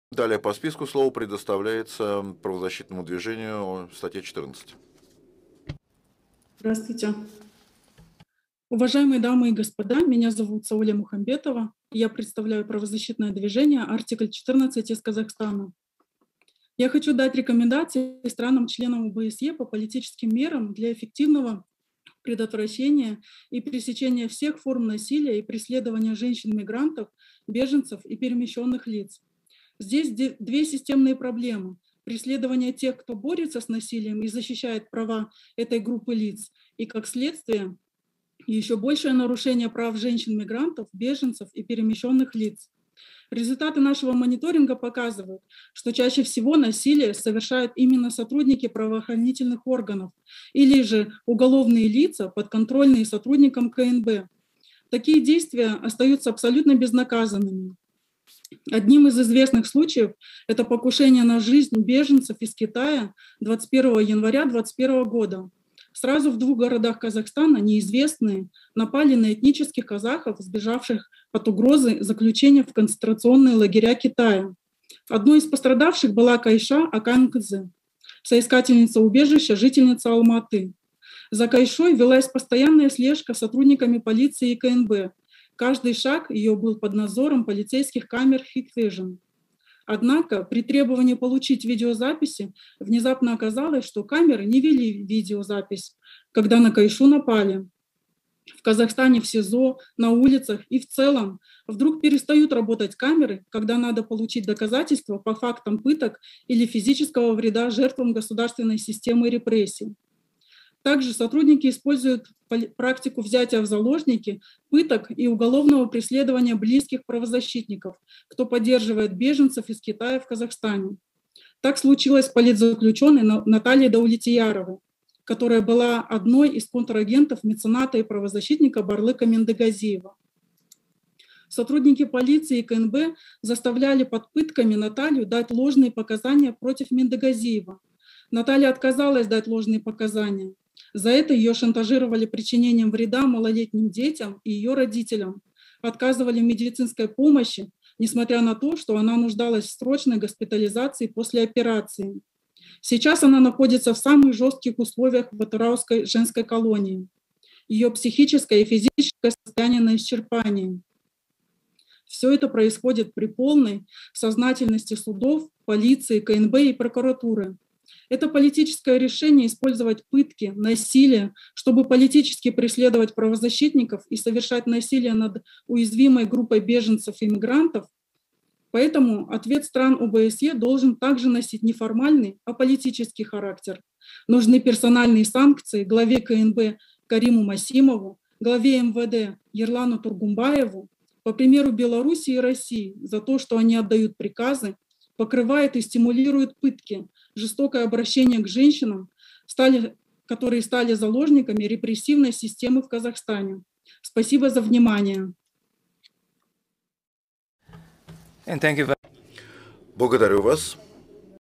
Выступление правозащитницы Сауле Мухамбетова на сессии ОБСЕ, 17.11.2021 г.
16-17 ноября  2021 года  в Варшаве прошел  семинар (в онлайн режиме),  организованный бюро ОБСЕ по демократическим институтам и правам человека.

Тема семинара: Предотвращение и борьба с насилием в отношении женщин и девочек: семинар по человеческому измерению.

На сессиях принимали участие правозащитники ассоциации

Публикуем видео и текст выступления правозащитницы правозащитного движения "Артикль 14" Сауле Мухамбетова на второй сессии

Уважаемые дамы и господа, меня зовут Сауле Мухамбетова и я представляю правозащитное движение Артикль 14 из Казахстана.
Я хочу дать рекомендации странам-членам ОБСЕ по политическим мерам для эффективного предотвращения и пресечения всех форм насилия и преследования женщин-мигрантов, беженцев и перемещенных лиц.

Здесь две системные проблемы: преследование тех, кто борется с насилием и защищает права этой группы лиц и,  как следствие, еще большее нарушение прав женщин-мигрантов, беженцев и перемещенных лиц.

Результаты нашего мониторинга показывают, что чаще всего насилие совершают именно сотрудники правоохранительных органов или же уголовные лица, подконтрольные сотрудникам КНБ. Такие действия остаются абсолютно безнаказанными.

Один из самых известных случаев - это покушения на жизнь беженцев из Китая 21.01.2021. Сразу в двух городах Казахстана «неизвестные» напали на этнических казахов, сбежавших от угрозы заключения в концентрационные лагеря Китая. Одной из пострадавших была Кайша Аканкызы, соискательница убежища, жительница Алматы. За Кайшой велась постоянная слежка сотрудниками полиции и КНБ, каждый шаг ее был под надзором полицейских камер Хиквижен. Однако при требовании получить видео записи внезапно оказалось, что камеры не вели видео запись, когда на Кайшу напали. В Казахстане в СИЗО, на улицах и в целом “вдруг перестают работать камеры”, когда надо получить доказательства по фактам пыток или физического вреда жертвам государственной системы репрессий.

Также сотрудники полиции используют практику взятия в заложники, пыток и уголовного преследования близких правозащитников, кто поддерживает беженцев из Китая в Казахстане. Так случилось с политзаключенной Натальей Даулетьяровой, которая была одной из контрагентов мецената и правозащитника Барлыка Мендыгазиева.

Сотрудники полиции и КНБ, заставляли под пытками Наталью дать ложные показания против Мендыгазиева. Наталья отказалась дать ложные показания. За это ее шантажировали причинением вреда малолетним детям и ее родителям, отказывали в медицинской помощи, не смотря на то, что она нуждалась в срочной госпитализации после операции.  Сейчас она находится в самых жестких условиях в Атырауской женской колонии. Ее психическое и физическое состоянии на исчерпании.

Все это происходит при полной сознательности судов, полиции, КНБ и прокуратуры. Это политическое решение использовать пытки, насилие, чтобы политически преследовать правозащитников и совершать насилие над уязвимой группой беженцев/мигрантов.  Поэтому ответ стран-ОБСЕ должен также носить не формальный, а политический характер.

Нужны персональные санкции к главе КНБ Кариму Масимову, главе МВД  Ерлану Тургмбаеву по примеру Беларуси и России за то, что они отдают приказы, покрывают и стимулирует пытки, жестокое обращение к женщинам, которые стали заложниками репрессивной системы в Казахстане.

Спасибо!